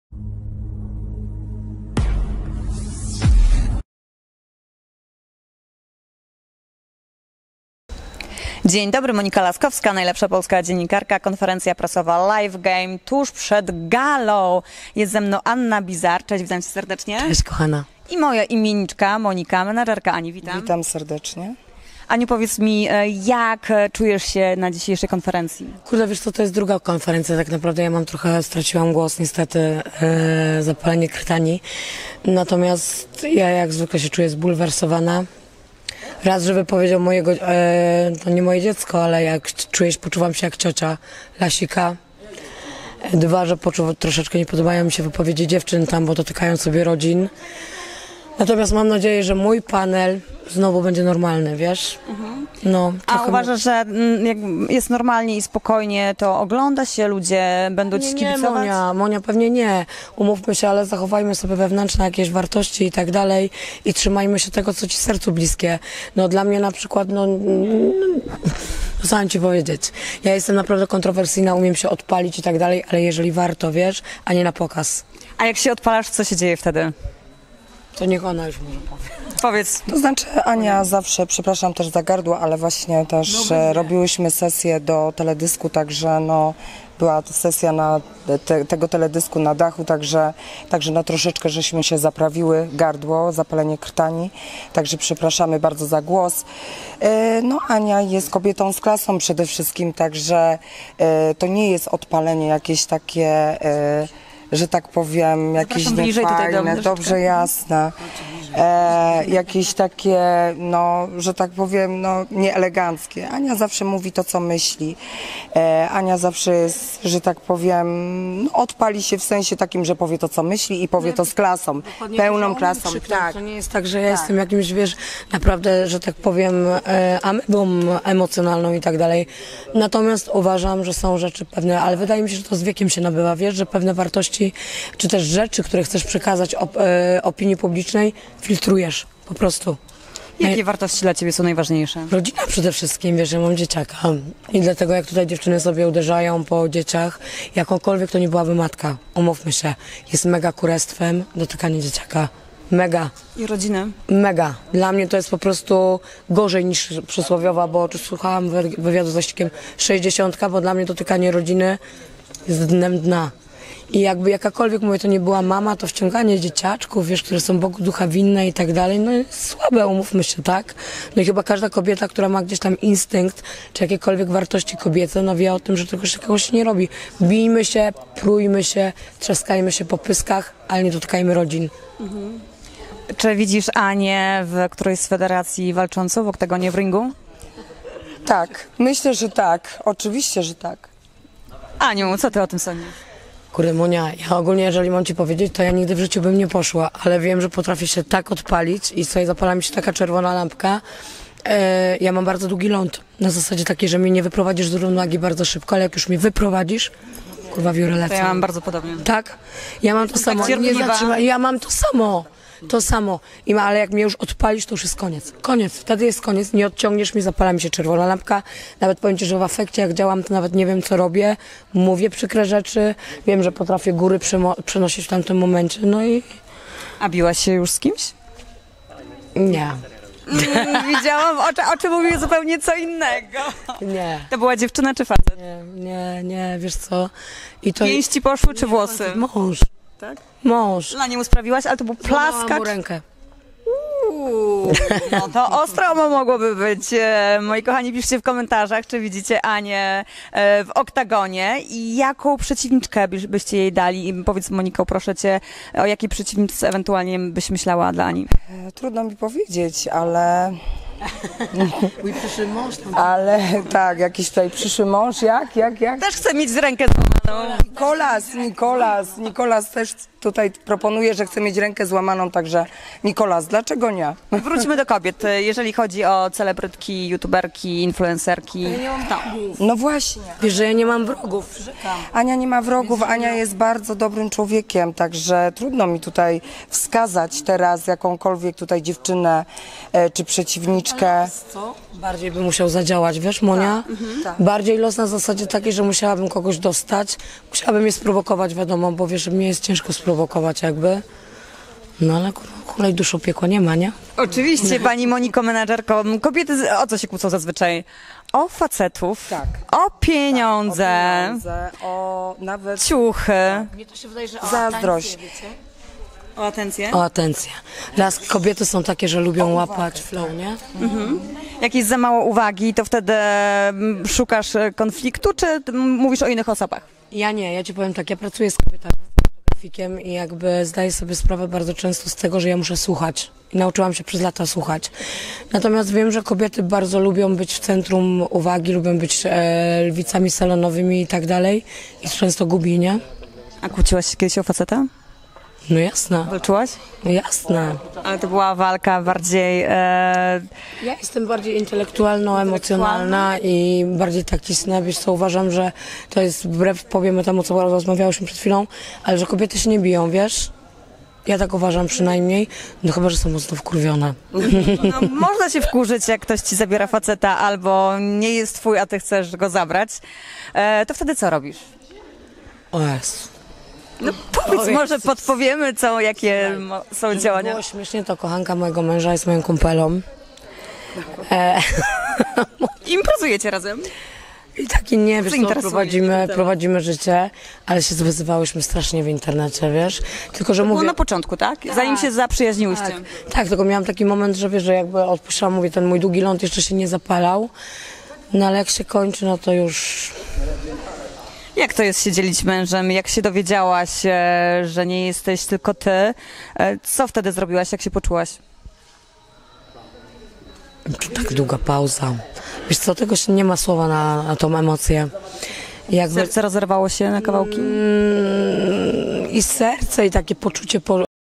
Anna Bizar: kochanka mojego męża jest moją kumpelą!
Mała Ania i Artur Kornik Sowiński na konferencji Live Game

Camera/Montaż
Instagram : krystian_smialek @Najlepsza Polska Dziennikarka